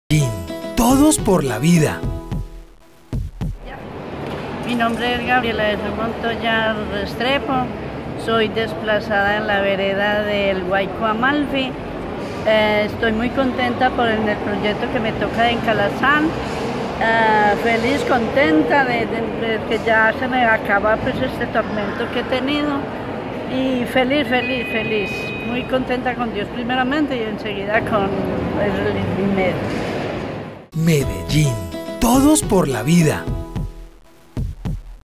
Familias beneficiarias de Altos de Calasanz II
En medio de risas y llanto de alegría, las familias beneficiarias del proyecto habitacional Altos de Calazans II conocieron la ubicación de sus nuevos hogares, donde junto a sus familias seguirán ConstruyendoSueños y muchas más oportunidades para la vida.